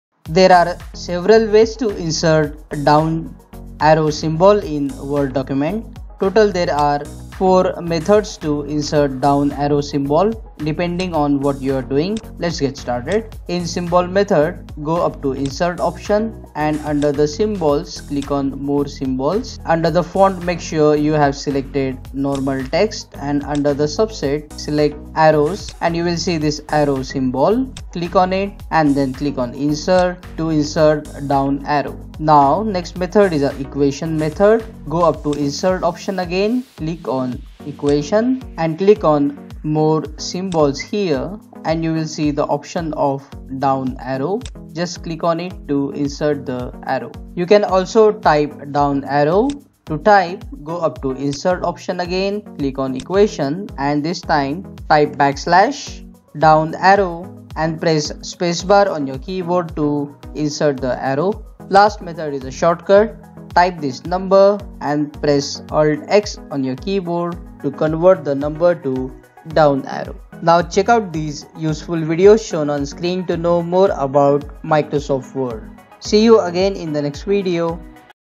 How to Insert Down Arrow Symbol In Word - [ ↓ ]
how to insert down arrow symbol in word document is shown in this video. You can also type down arrow by keyboard too after watching this video. In general down arrow symbol in chemistry is used to show precipitate (ppt) in a chemical reaction.
Down arrow copy and paste from here: ↓